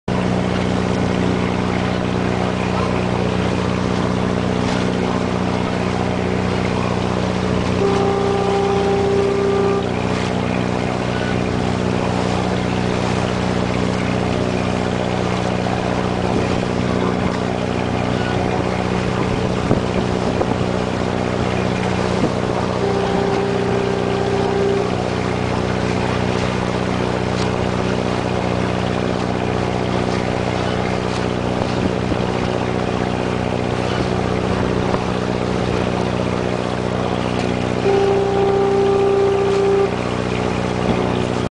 Ship John Shoal Light
Passing Ship John Shoal Lighthouse / Delaware Bay, NJ